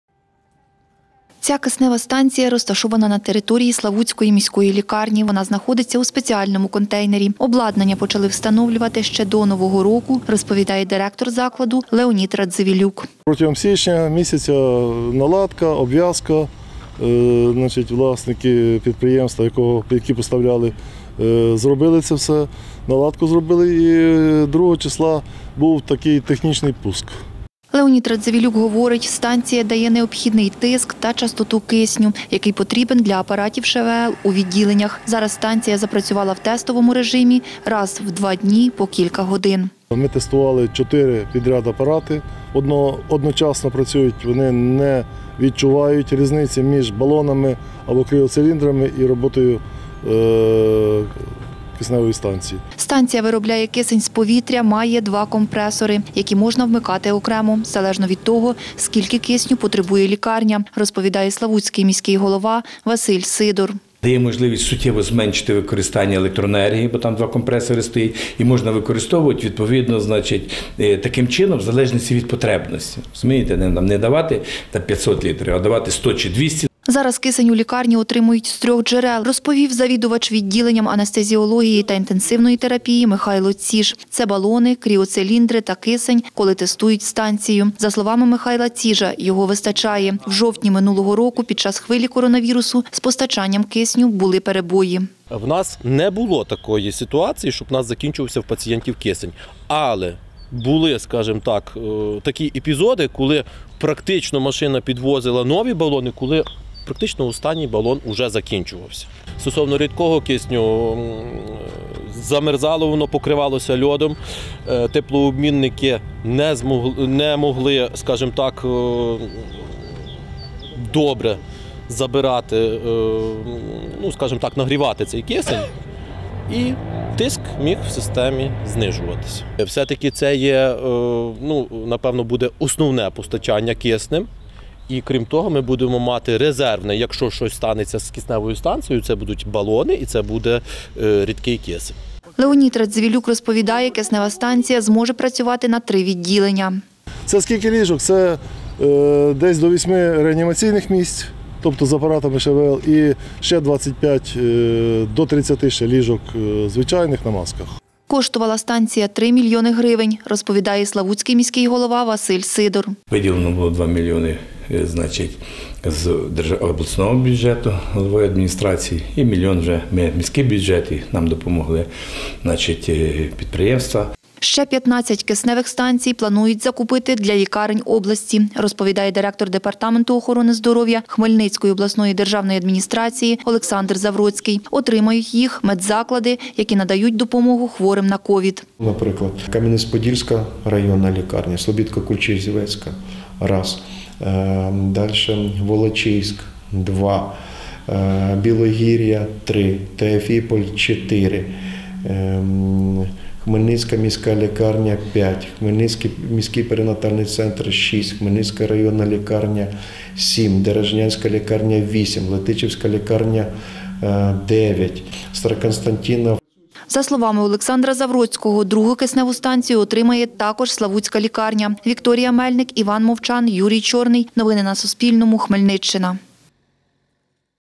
У Славутській міській лікарні запрацювала киснева станція в тестовому режимі
В тестовому режимі запрацювала киснева станція у Славутській міській лікарні. Про це розповів директор закладу Леонід  Радзивілюк. За його словами, станція виробляє кисень з повітря, має два компресори різної потужності, їх можна залучати залежно від того, скільки потребуватиме лікарня кисню. Як каже Леонід Радзивілюк, така станція зможе працювати на 3 відділення. Детальніше дивіться далі.